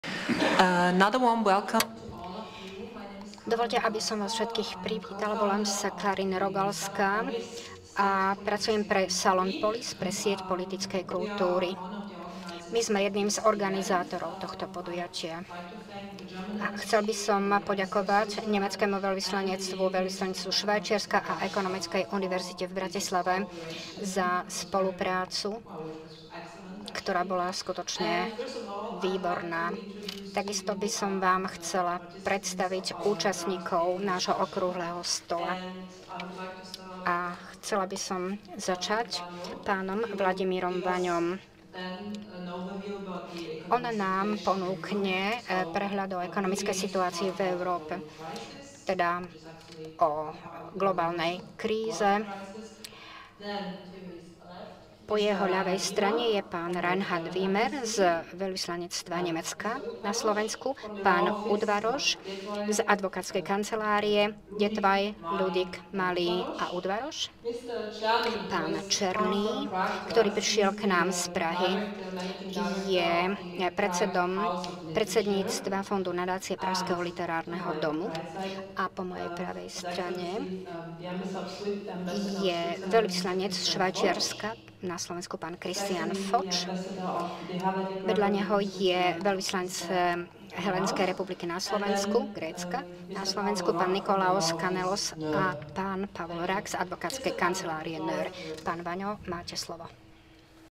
Kritický siedmy rok - Predstavenie
Kritický siedmy rok.
Zatažkávacie skúšky vo vztahoch medzi Slovenskom a Európskou úniou.
Sedem rokov uplynulo od vstupu Slovenska do Európskej únie. Vo vztahoch medzi ludmi sa
siedmy rok považuje za klúcový. Platí to v case krízy v Eurozóne rovnako na úrovni štátov?
Osem presvedcených Európanov bude o tomto diskutovat v rámci okrúhleho stola, na ktorý
Vás srdecne pozývame.
Miesto: Ekonomická univerzita v Bratislave, Dolnozemská cesta 1
Spolocenská miestnost V-2, nová budova, prízemie.
Dátum a cas: 28. marec 2012, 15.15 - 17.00 hod.
Program:
1. Privítanie (Jana Lenghardtová, Ekonomická univerzita v Bratislave, prorektorka pre
zahranicné vztahy)
2. Úvod: Súcasné výzvy pre Eurozónu (Vladimír Vano, Volksbank Slovensko, Bratislava,
hlavný analytik)
3. Okrúhly stôl: Kritický siedmy rok. Zatažkávacie skúšky vo vztahoch medzi Slovenskom
a Európskou úniou.
Diskutujúci:
Christian Fotsch, velvyslanec Švajciarskej konfederácie na Slovensku
Nicolaos D. Kanellos, velvyslanec Helénskej republiky na Slovensku
Reinhard Wiemer, zástupca velvyslanca Spolkovej republiky Nemecko na Slovensku
František Cerný, predseda predsedníctva Fondu nadácie Pražského literárneho domu
Pavol Rak, advokátska kancelária Noerr s.r.o., Bratislava
Vladimír Vano, Volksbank Slovensko, Bratislava, hlavný analytik
Ladislav Udvaros, partner advokátskej kancelárie Detvai Ludik Malý Udvaros, Bratislava
Moderátorka:
Karin Rogalska, Salon Polis -- siet politickej kultúry
4. Diskusia s verejnostou
5. Caša vína